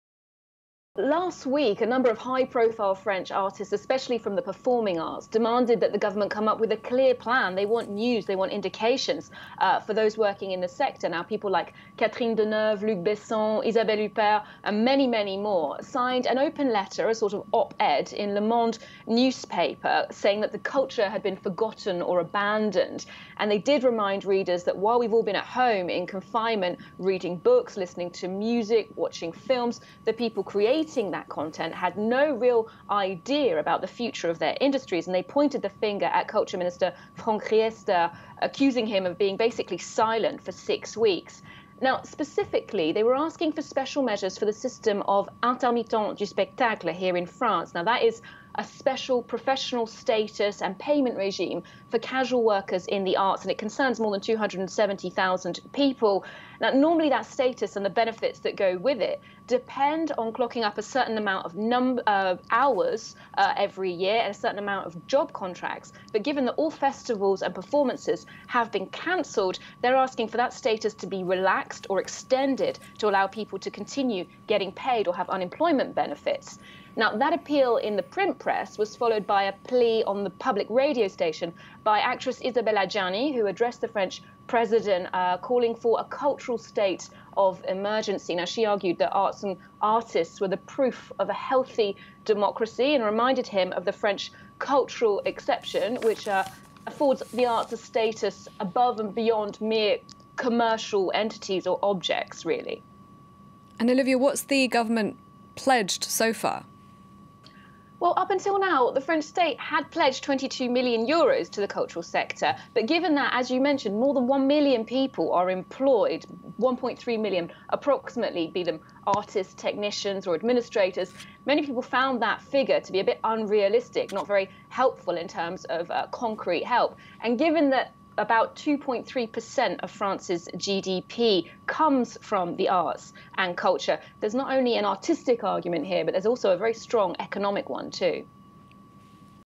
French arts sector facing 'cultural state of emergency'
French President Emmanuel Macron said Wednesday he was looking into more financial aid for the country's arts and culture sectors that have been hit hard by the coronavirus pandemic. France 24's culture editor Olivia Salazar-Winspear explains.